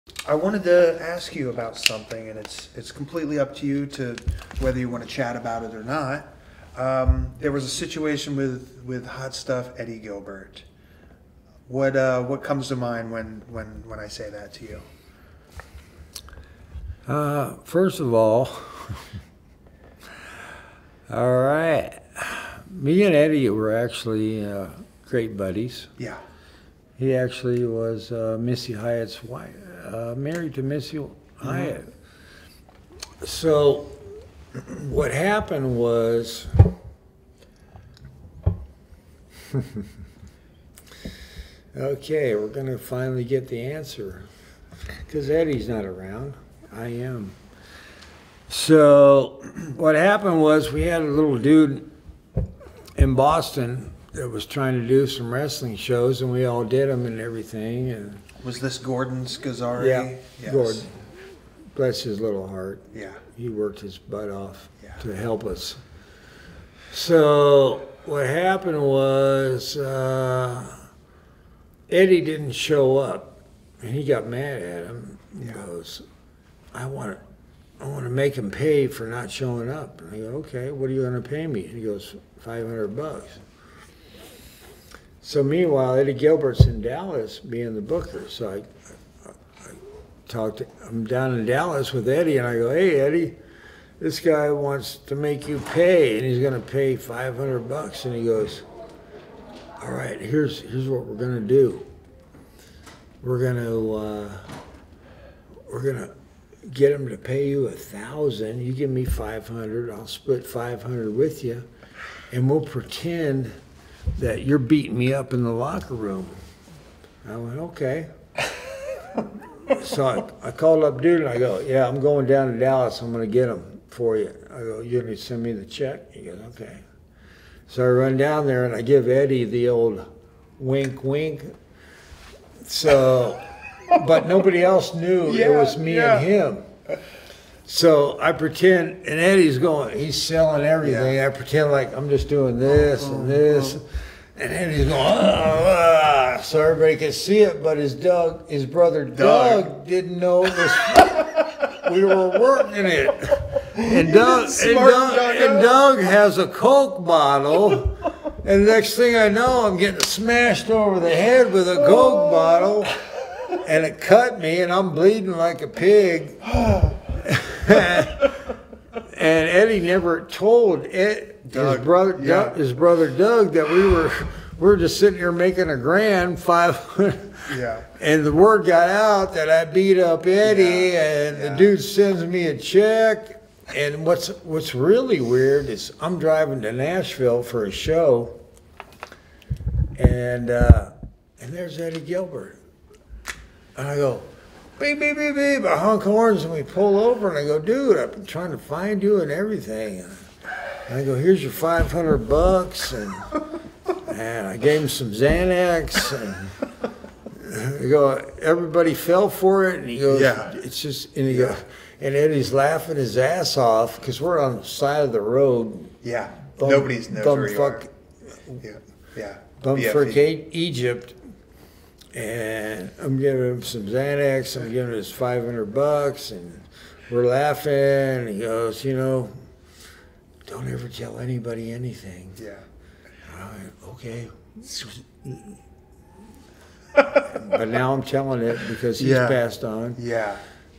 Jeff Gaylord Shares Eddie Gilbert Fight Story
Stream the full shootconversation with Chris Hero on the HSWN!